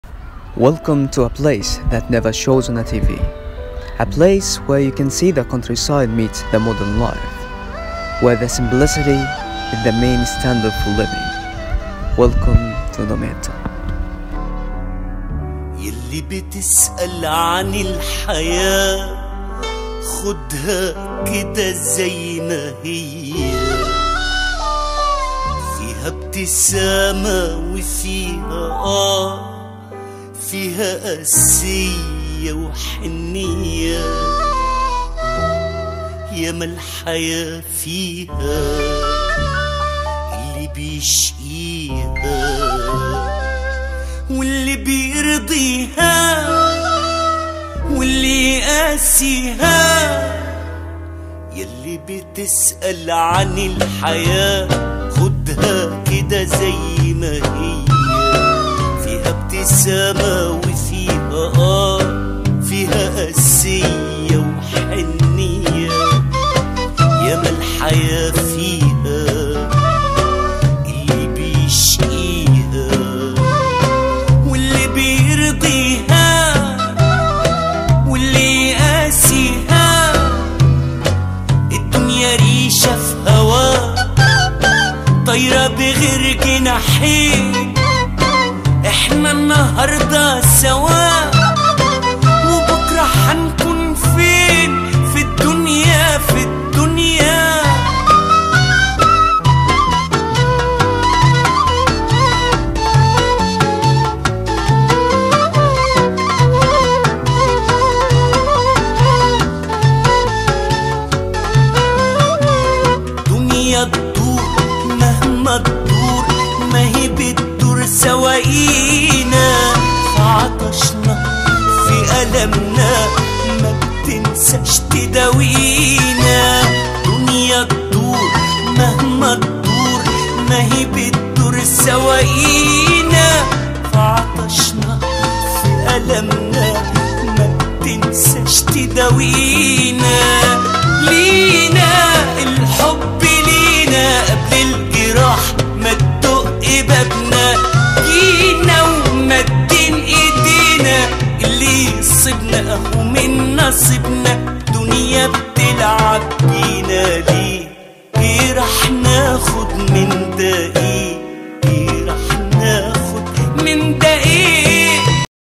Where the Nile meets the Mediterranean Damietta t دمياط عندما يتلاقى النيل مع البحر
In this video you will see the beauty of the simple life of the modern life mixing with the countryside the three cities inside Damietta and the beauty of them